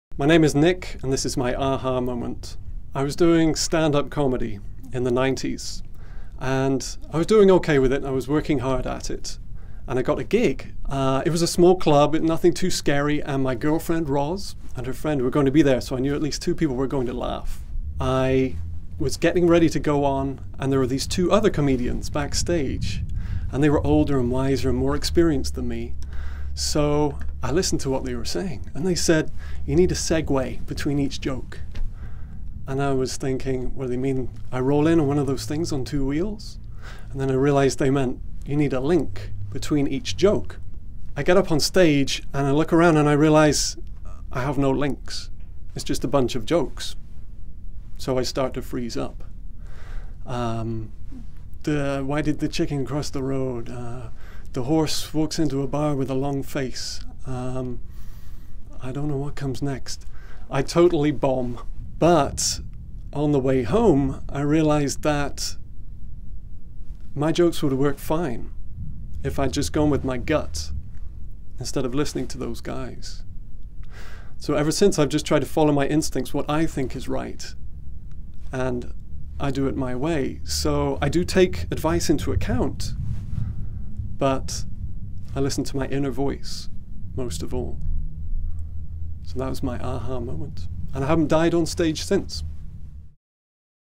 Nick's aha moment
I am an actor and comedian. I remember at one event I was supposed to perform a stand up comedy routine. Before I went on two fellow comedians who had been in the business forever gave me some advice. They said that I needed to have more segues between my jokes. Through my entire routine, I was focused more on making good segues than on the timing of my jokes and I totally bombed. That was they day that I learned that you don't always have to take the advice people give you.